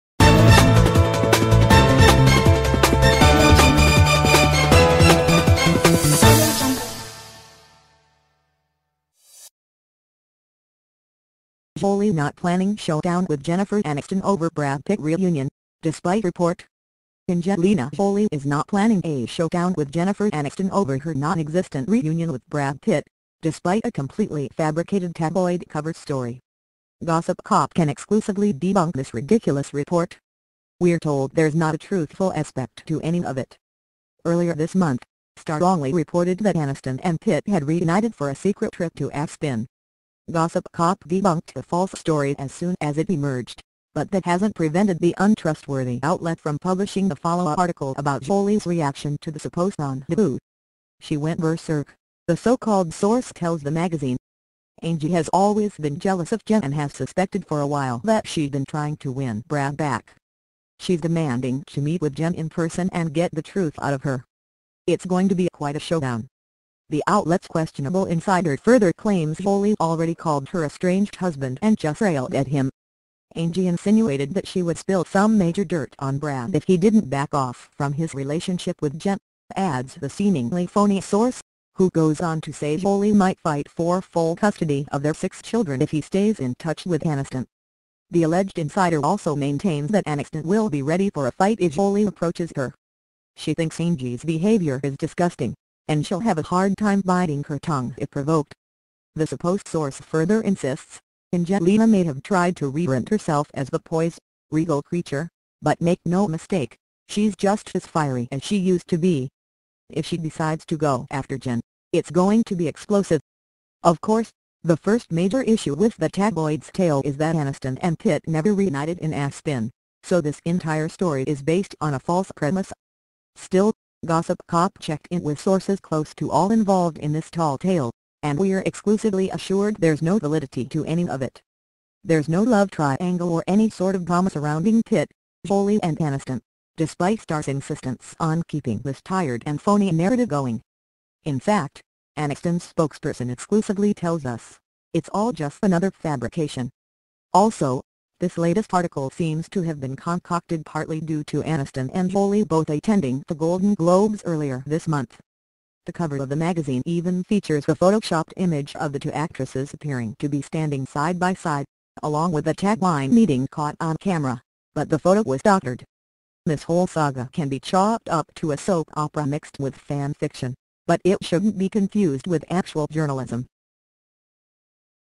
Angelina Jolie NOT planning “challenge” with Jennifer Aniston over Brad Pitt reunion
Angelina Jolie is not planning a “challenge” with Jennifer Aniston over her reunion with Brad Pitt, despite a completely fabricated tabloid cover story